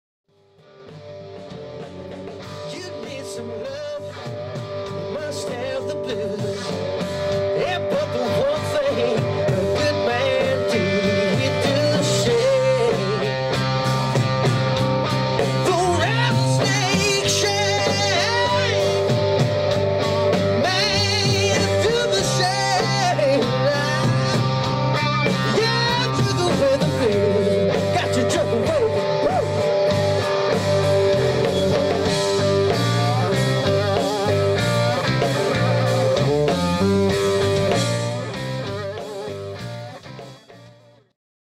Ross Street Plaza, Salmon Arm
Jimmy Bowskill plays at the Ross Street Plaza in downtown Salmon Arm.  At the age of 11 he was invited to join Jeff Healey onstage and now a whirlwind of activity has been bestowed on him with major festivals, radio and television appearances.  The residents of Salmon Arm, BC still talk about his fantastic performances at the Roots and Blues Festival.  Also check out for more information.